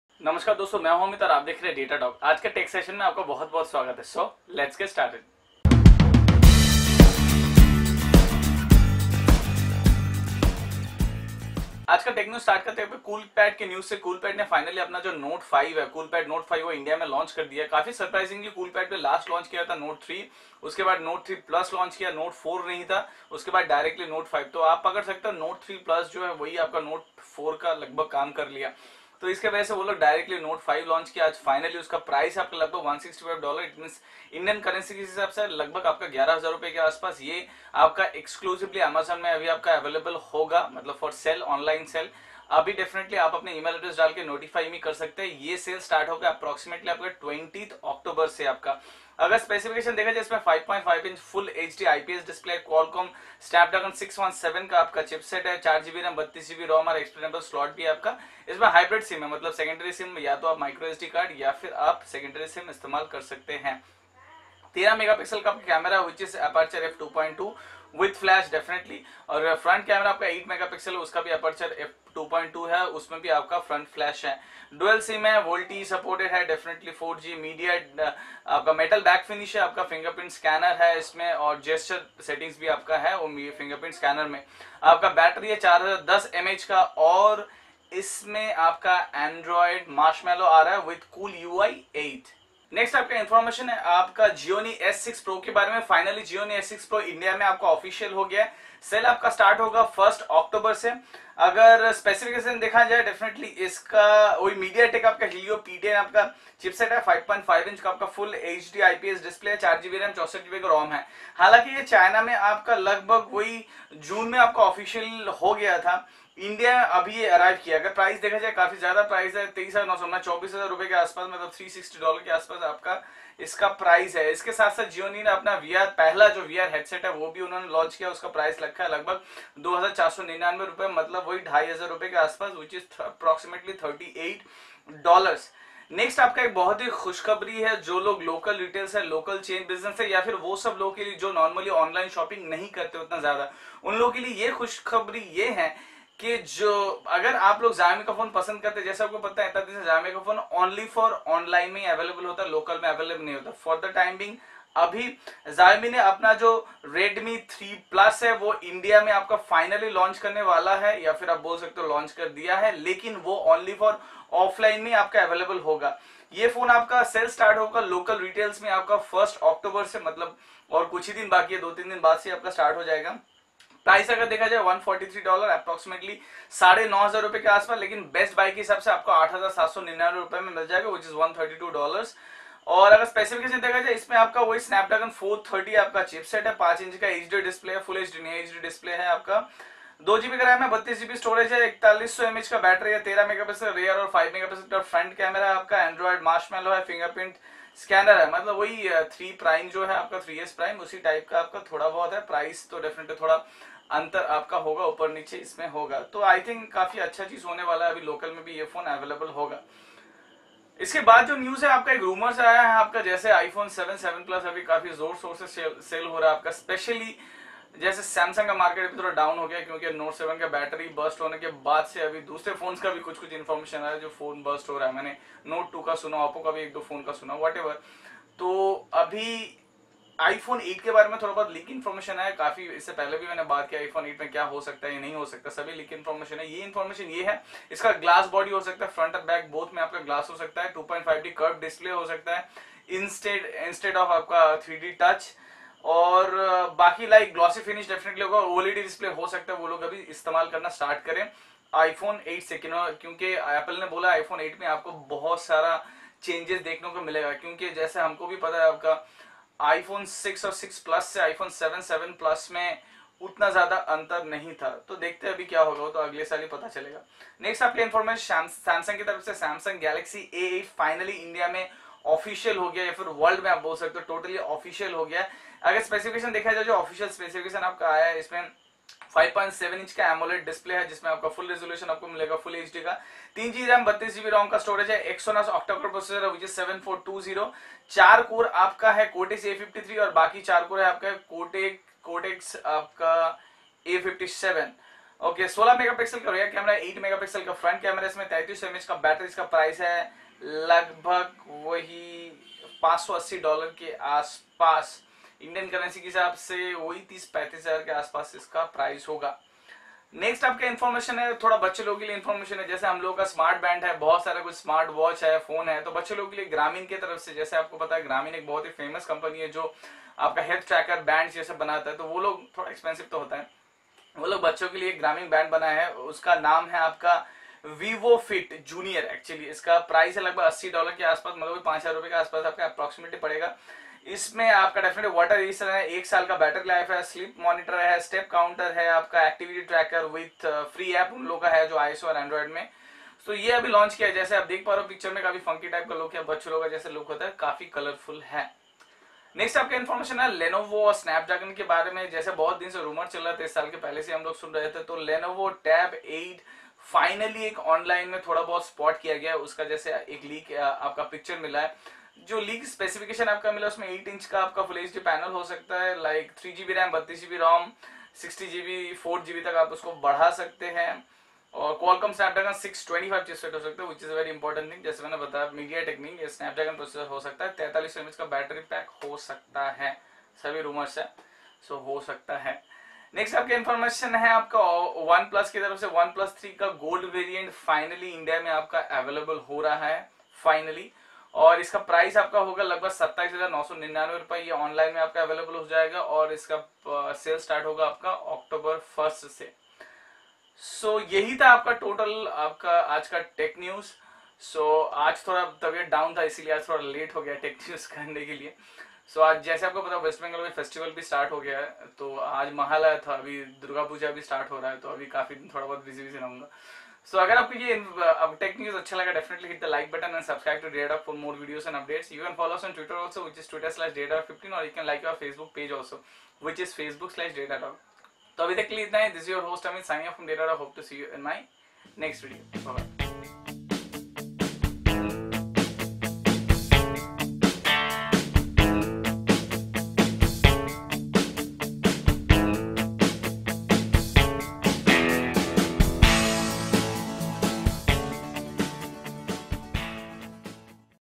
Tech News #31 - Coolpad Note 5,Gionee S6 Pro,Xiaomi Redmi 3S Plus,iPhone 8,Samsung Galaxy A8
Thanks,
Data Dock